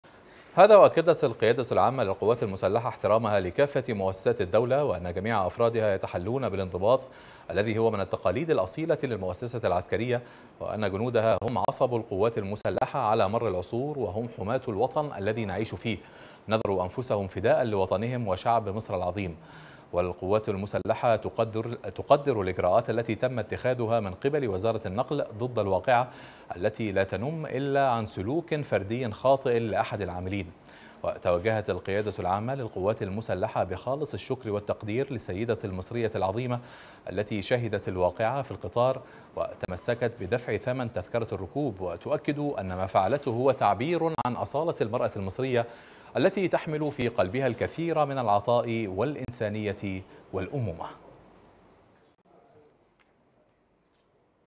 الآن | القوات المسلحة: جنودنا يتحلون بالانضباط ونشكر السيدة المصرية العظيمة التي شهدت واقعة القطار
الآن |  القيادة العامة للقوات المسلحة: جنودنا يتحلون بالانضباط ونشكر السيدة المصرية العظيمة التي شهدت واقعة القطار